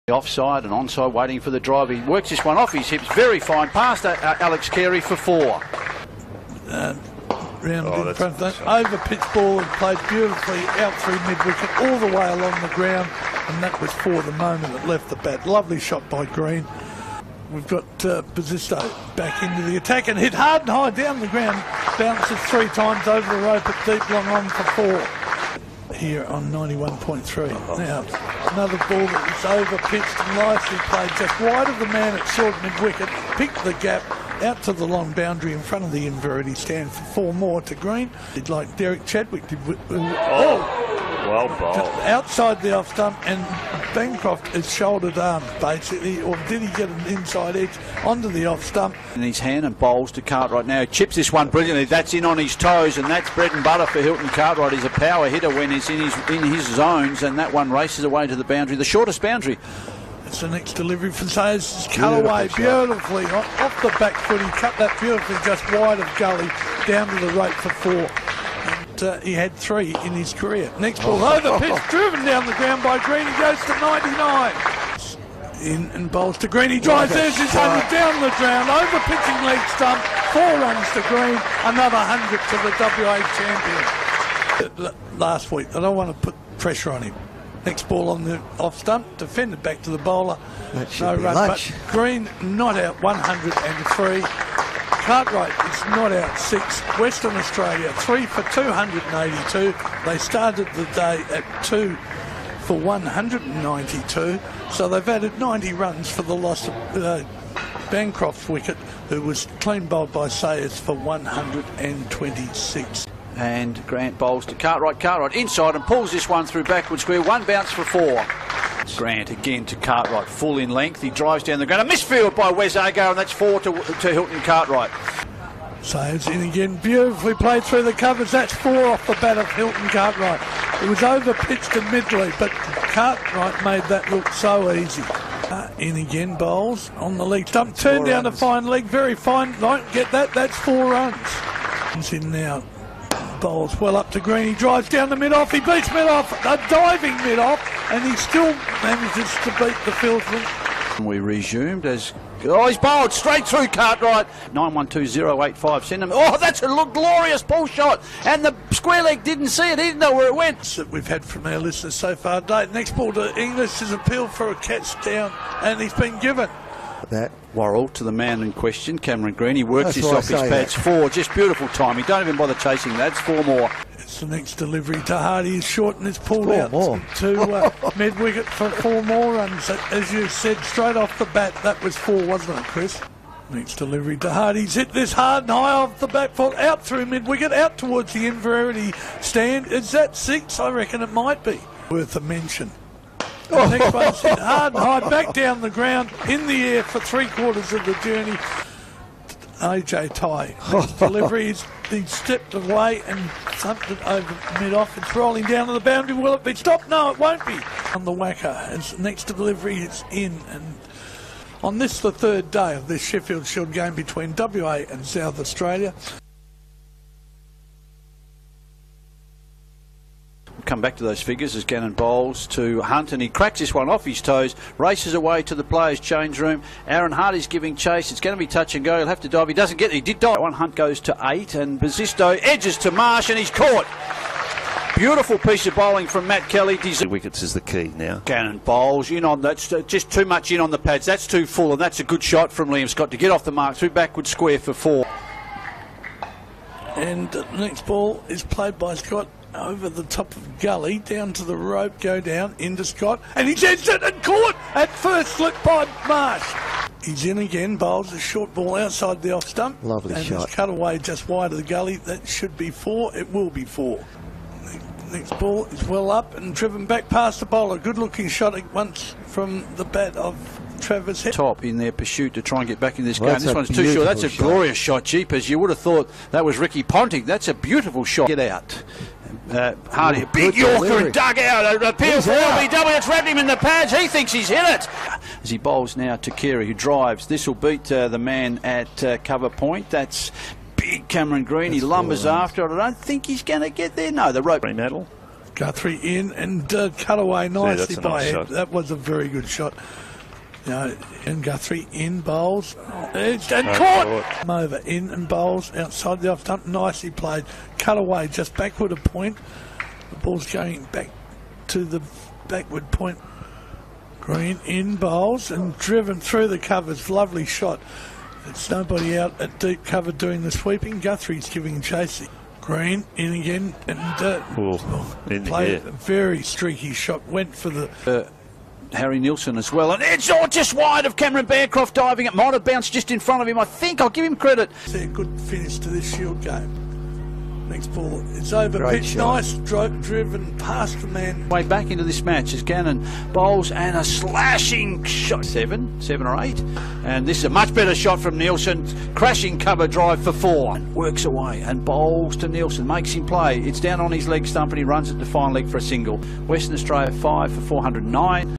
Redbacks build strong lead after Green's unbeaten 168
Alex Carey and Harry Nielsen's unbroken fifth-wicket stand ensured South Australia would sleep well heading into day four, with the visitors ahead by 243 at the WACA Ground